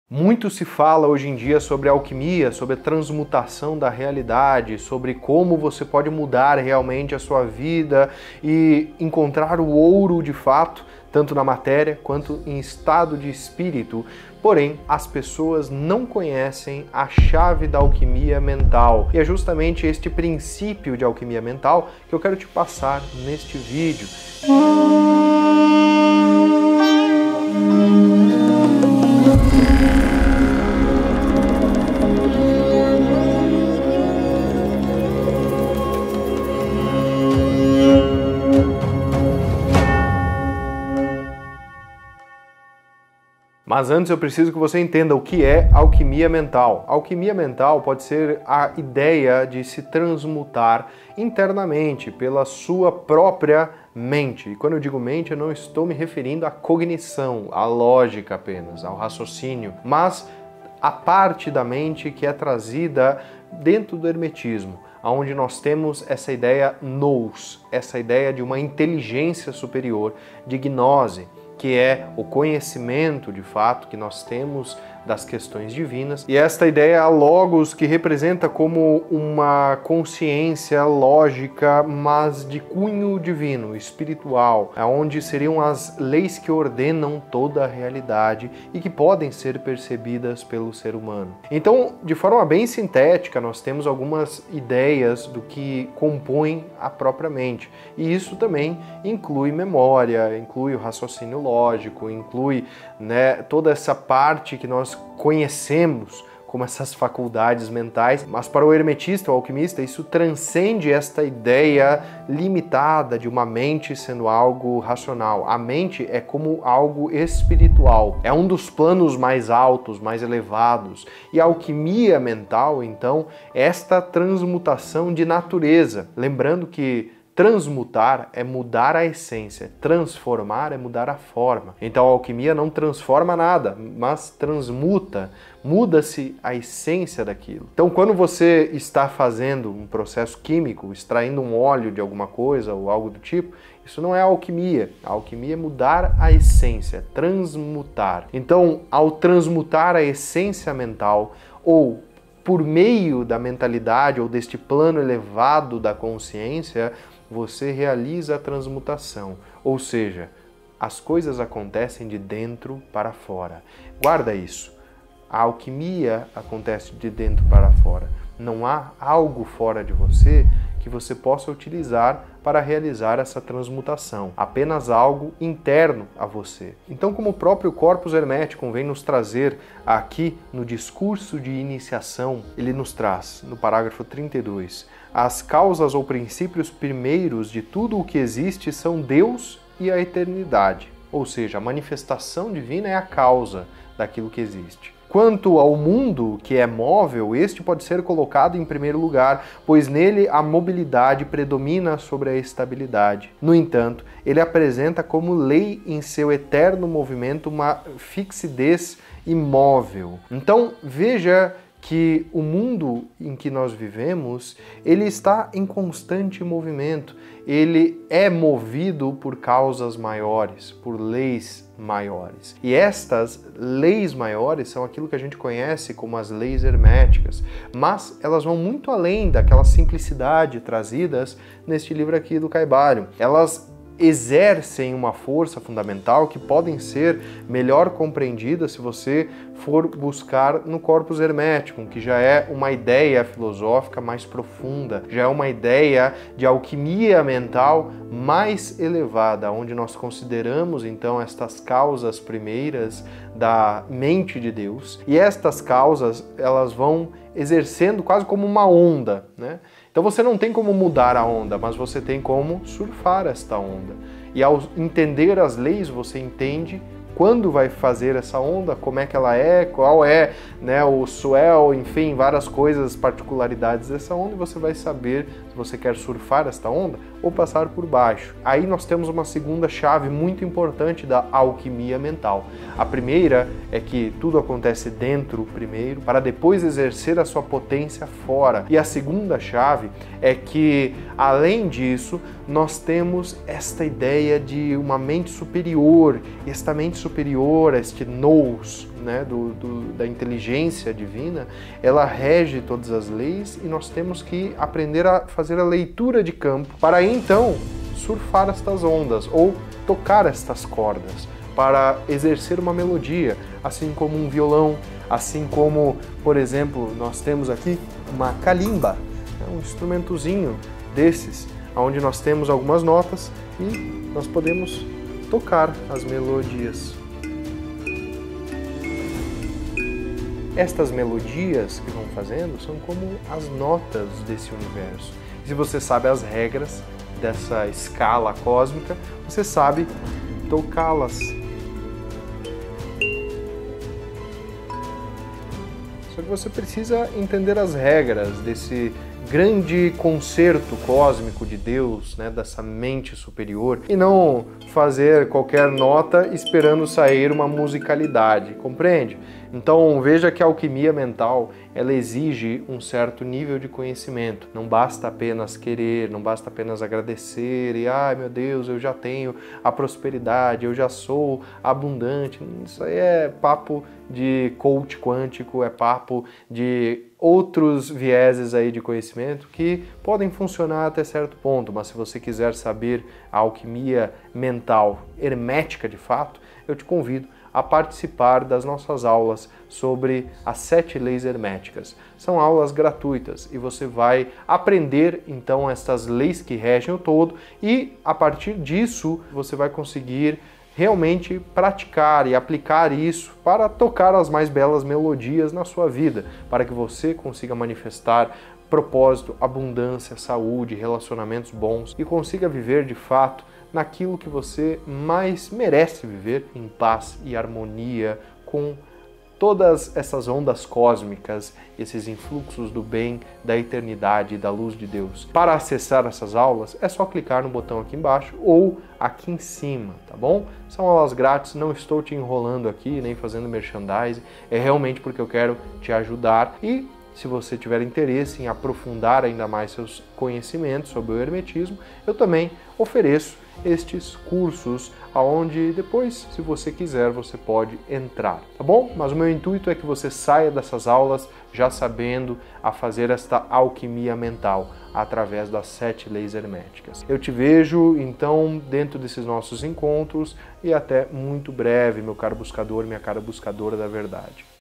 A CHAVE PARA A ALQUIMIA MENTAL FINALMENTE FOI ENCONTRADA!
👇🏼🗝 Adentre agora à Formação em Hermetismo & Alquimia 🔥👇🏼

Neste vídeo, eu te mostro como transformar pensamentos em poderosas poções para alcançar metas, superar desafios e alcançar a plenitude. Prepare-se para desvendar os mistérios da Alquimia Mental e embarcar em uma transformação única e enriquecedora. Sejam bem-vindos ao poder de uma mente extraordinária!

Vamos começar essa jornada juntos!

Sumário
00:00 - Introdução
00:43 - O que é Alquimia Mental?
2:55 - As Leis da Manifestação Superior
6:30 - UM CONVITE!

Compre por esses links e ajude o canal!
História da Magia
A Chave dos Grandes Mistérios
O Caibalion
Corpus Hermeticum